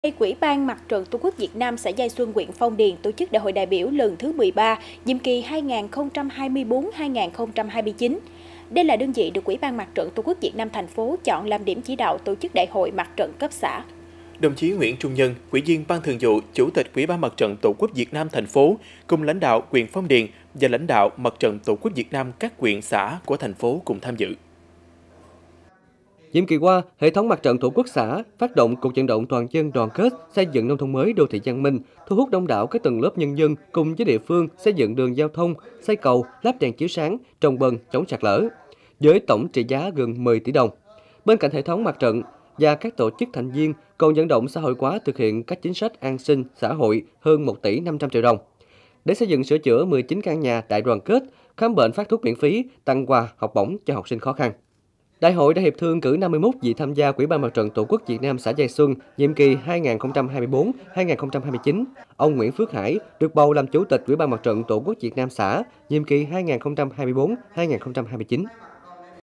Đại hội đại biểu MTTQVN điểm cấp xã của TP. Cần Thơ| Cần Thơ TV
UBMTTQVN xã Giai Xuân, huyện Phong Điền tổ chức Đại hội đại biểu lần thứ XIII, nhiệm kỳ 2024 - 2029. Đây là đơn vị được UBMTTQVN TP chọn làm điểm chỉ đạo tổ chức đại hội mặt trận cấp xã.